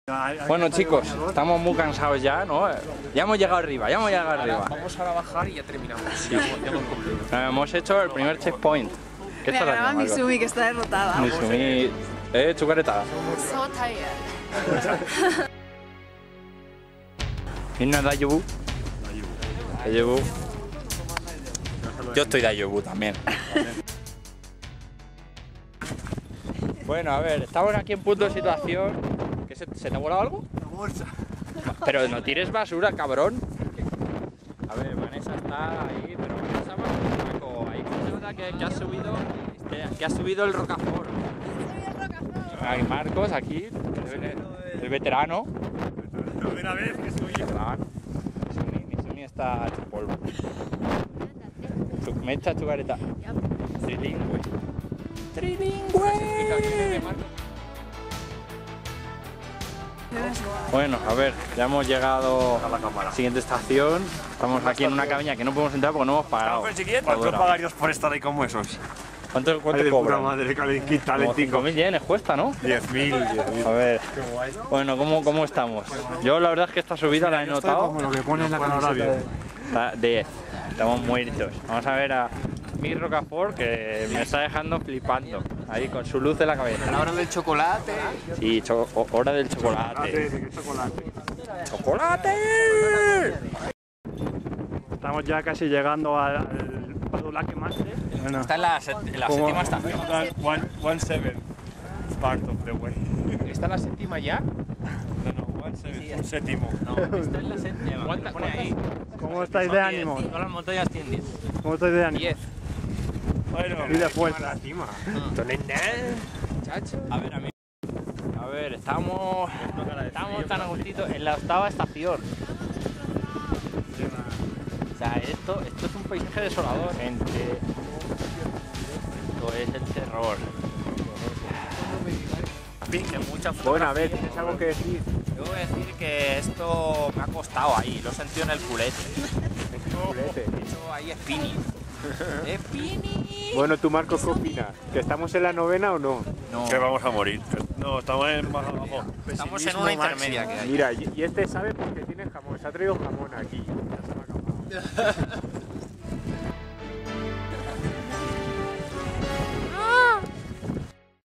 Subida nocturna al monte Fuji
Comienza la escalada: estaciones 6, 7 y 8